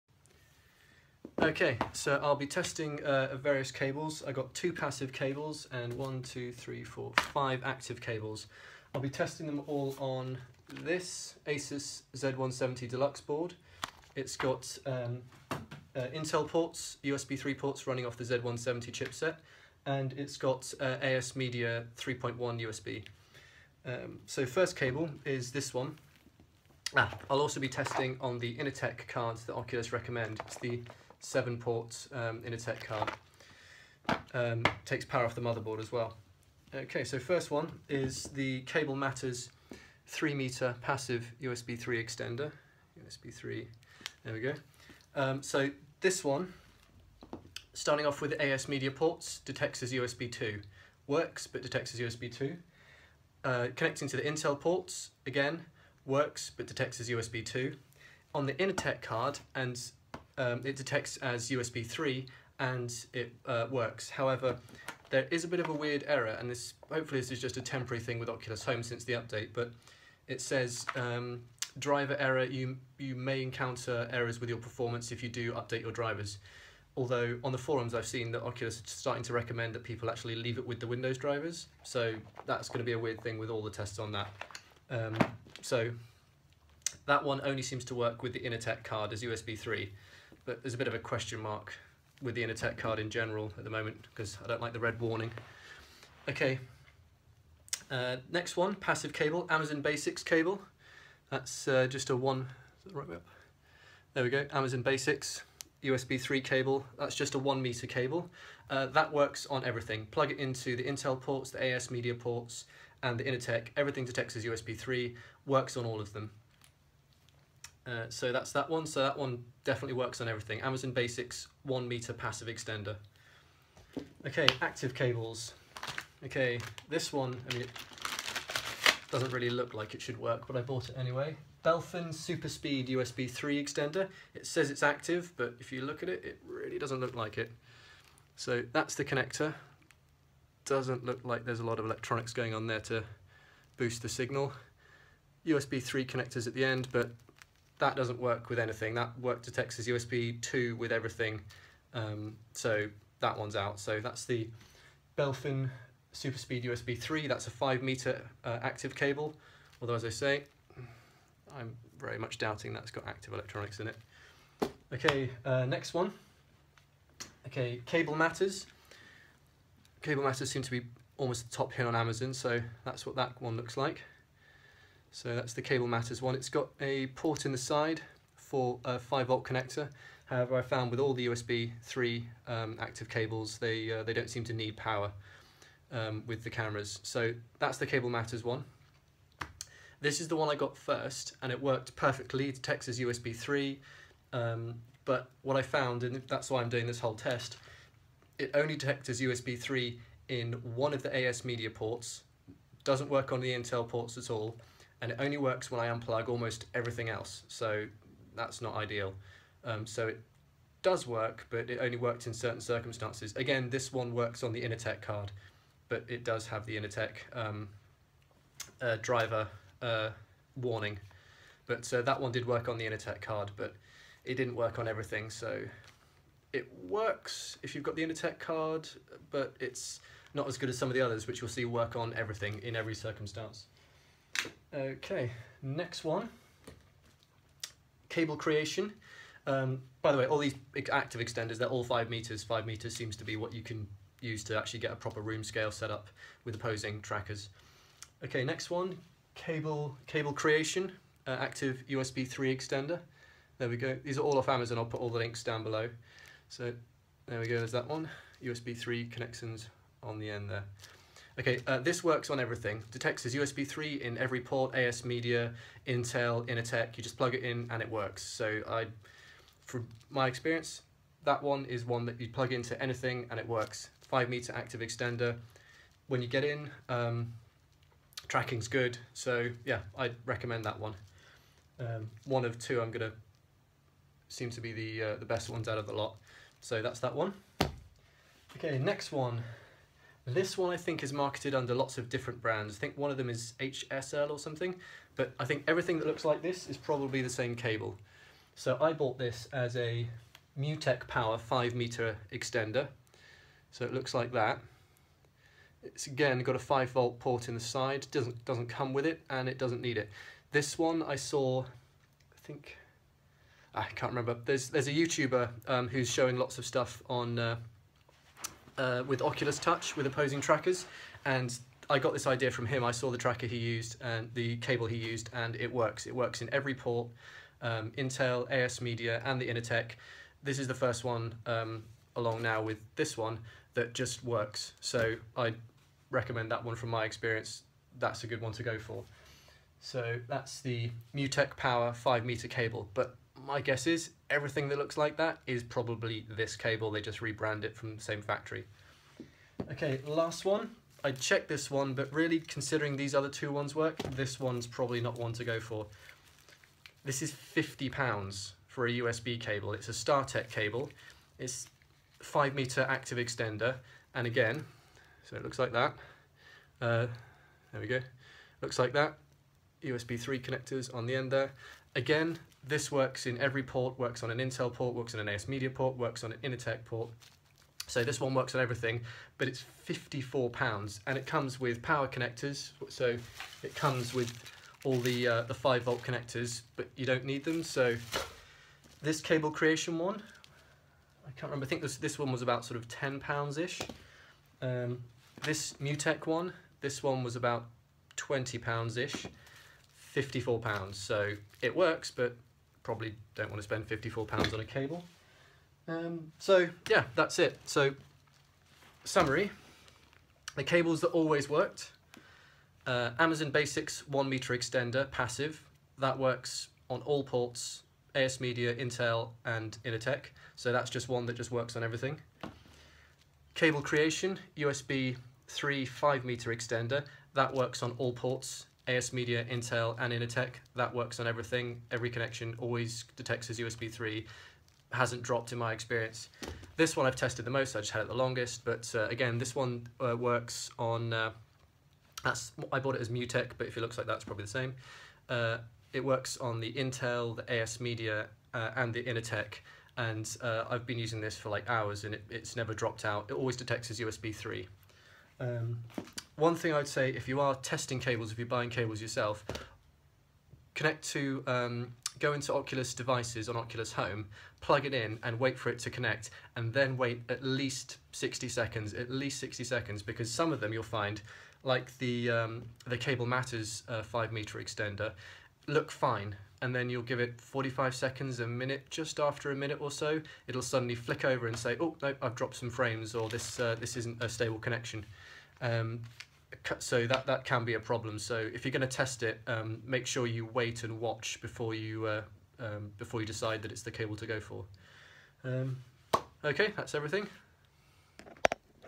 Oculus CV1 sensor USB3 extender group test
I did a group test on USB3 extenders here is what I found out :

I tested using an Asus z170 Deluxe motherboard it has both Intel and AsMedia USB3 ports.  I also used the Oculus recommended Inatek 7 port USB 3 Card.

——Worked——

Cables in this category mean the sensor detected as USB3 in Oculus devices and didn’t lose connection and provided solid tracking testing with the headset on, no matter what brand of USB port it was connected to (Intel, AsMedia,Inatek).

MutecPower 5m active USB3 extension cable £22

(picture seems to be wrong on amazon website)

CableCreation 5m active USB3 extension cable £12

Amazon Basics 1m Passive USB3 extension cable £4

——Sort of Worked——

Cable Matters 3m Passive USB3 extension cable £7

Detected as USB3 when using the InatekCard (which has extra power from a SATA power connector)
Detected as USB2 when using motherboard Intel or AsMedia USB3 ports

Cable Matters 5m Active USB3 extension cable £11

Only worked as USB 3 in one of the AsMedia ports and only when everything else was disconnected. Was USB2 or has an error in other AsMedia or Intel ports. Detected as USB3 in the Inatek Card and seemed ok testing in Oculus home loading area.

——Didn’t work——

Belfen Superspeed 5m active USB3 extension cable

detected as USB2 on every USB3 port including Inatek card